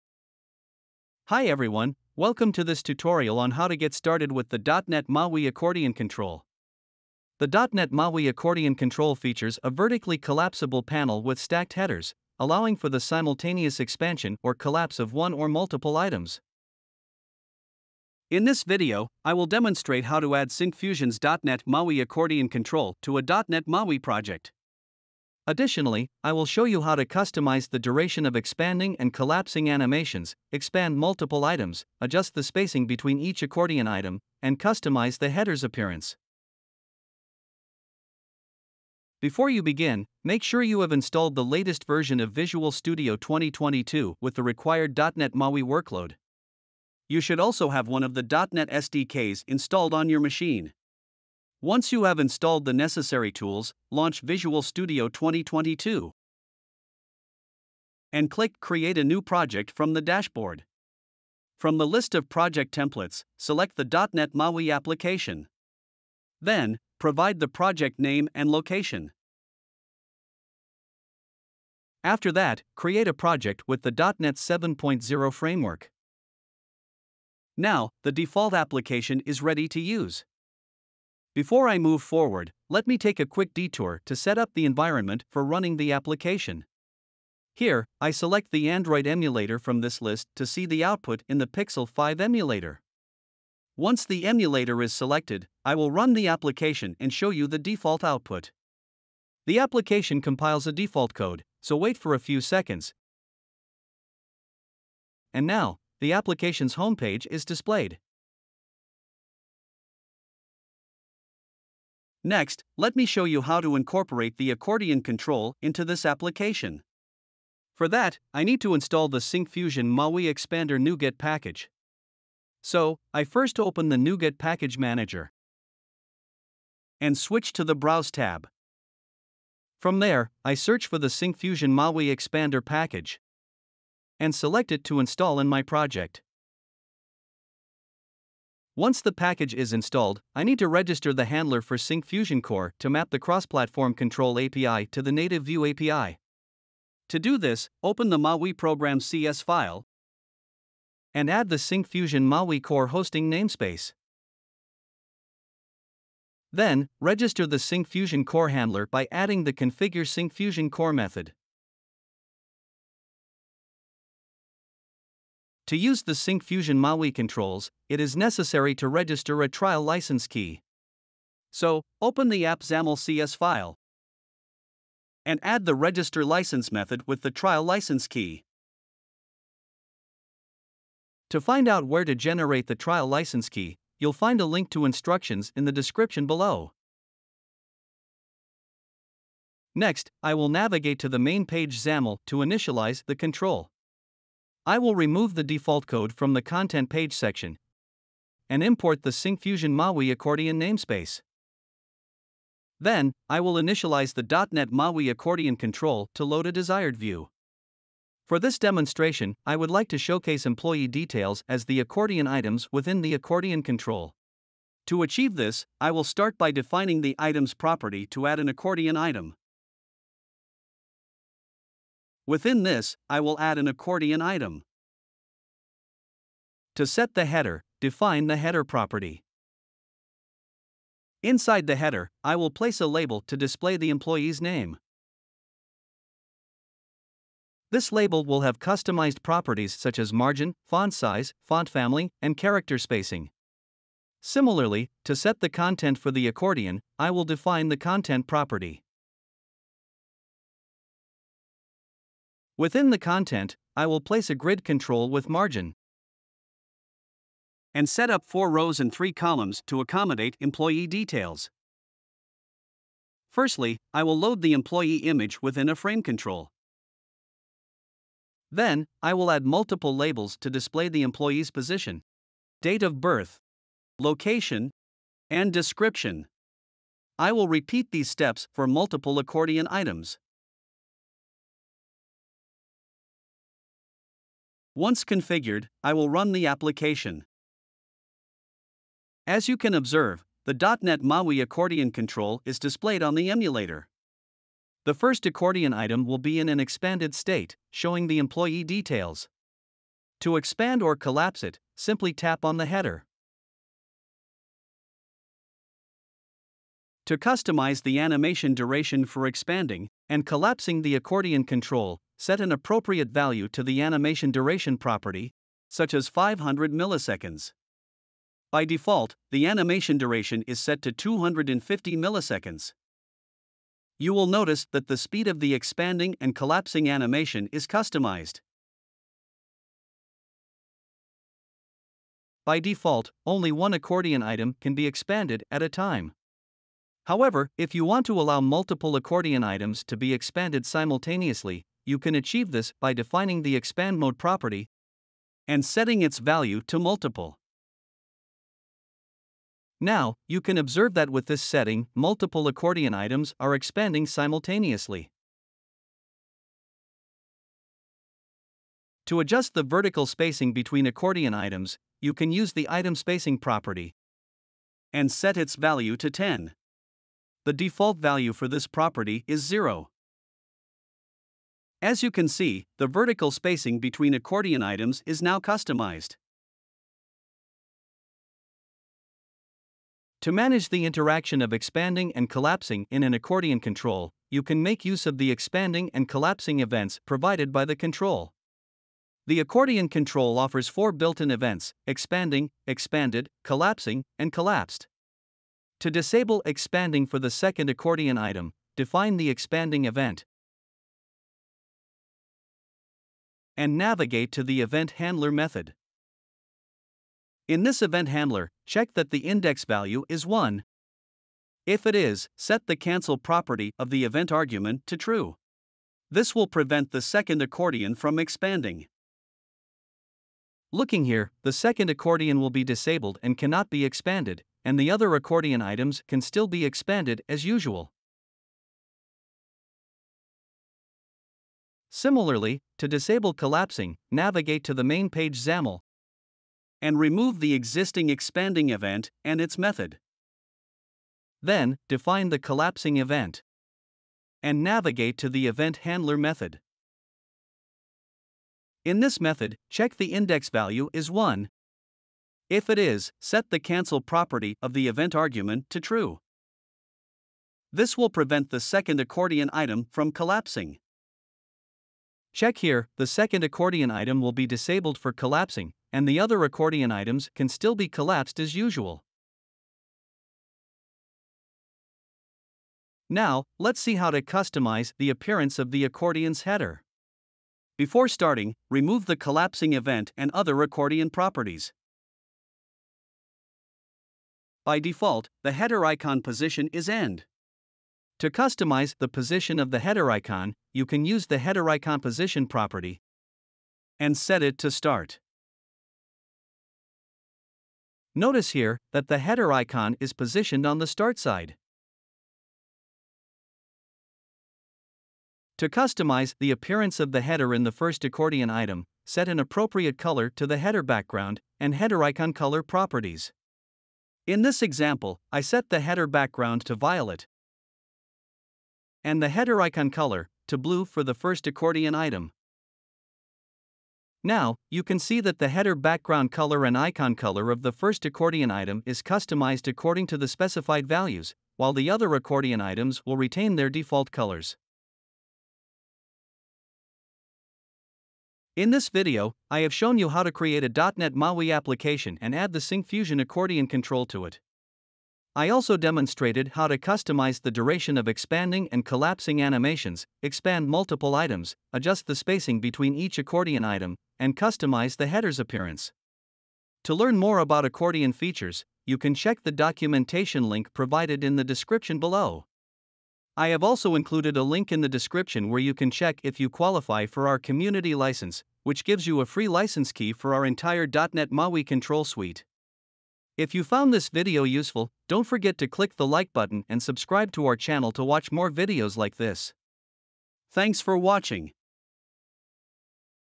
Getting Started with the .NET MAUI Accordion Control
Learn how to create and configure the Syncfusion .NET MAUI Accordion control in a .NET MAUI project using Visual Studio 2022. This control offers a vertically collapsible panel with stacked headers. It allows you to expand or collapse one or multiple items simultaneously to display related content.

In this video, you will learn how to add the .NET MAUI Accordion control to a .NET MAUI app and add multiple items to it. I also showed how to customize the duration of expanding and collapsing animations, expand multiple items, adjust the spacing between accordion items, and customize the header's appearance.

The Accordion control offers both single and multiple expansion modes. In the single mode, only one item can be opened at a time, whereas in the multiple mode, multiple items can be opened simultaneously. Users have the option to customize the vertical spacing between items. It also allows customization of the header icon placement.

If you need a trial license key, you can start a .NET MAUI trial from your Syncfusion account and then obtain a trial license key from the downloads

BOOKMARK DETAILS
[00:00] Introduction
[00:51] Create a MAUI app
[01:51] Add the MAUI accordion control
[05:06] Customize animation duration
[05:32] Expand multiple items
[05:58] Customize item spacing
[06:19] Disable the expand action for a particular item
[07:11] Disable the collapse action for a particular item
[07:52] Customize header appearance